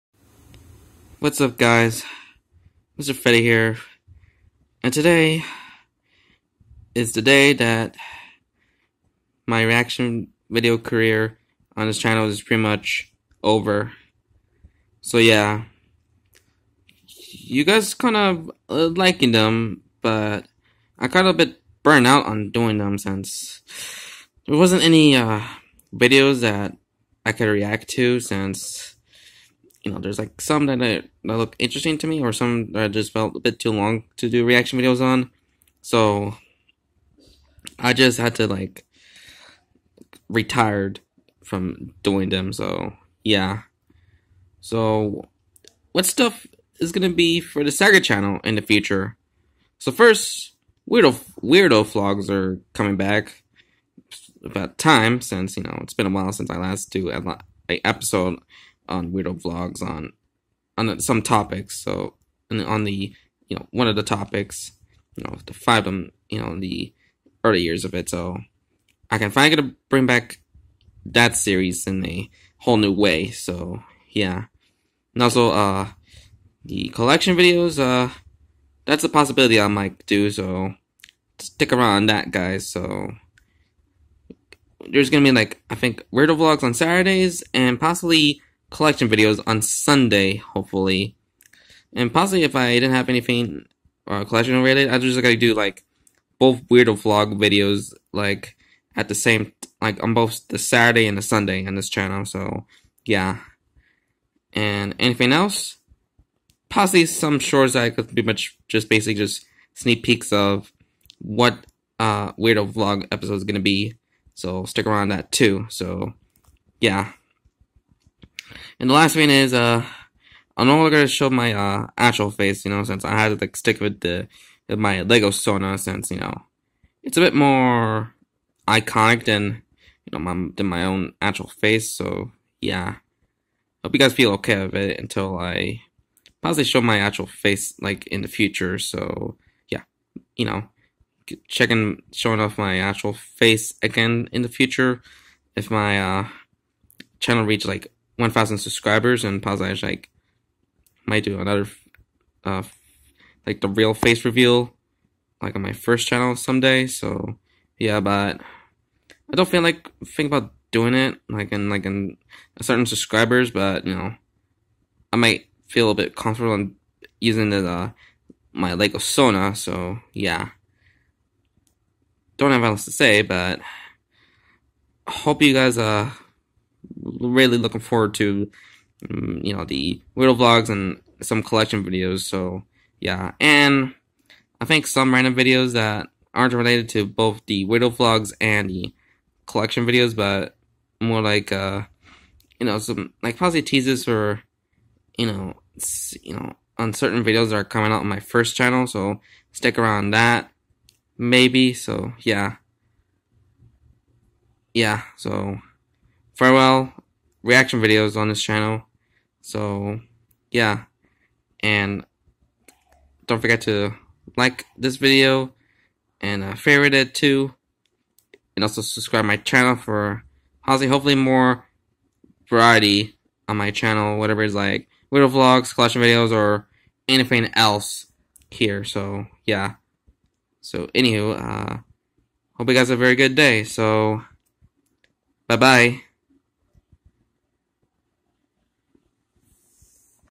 Farewell to reaction videos and the future of this channel
My career on doing reaction videos is over but there's more stuff that I want to do on this channel for more variety...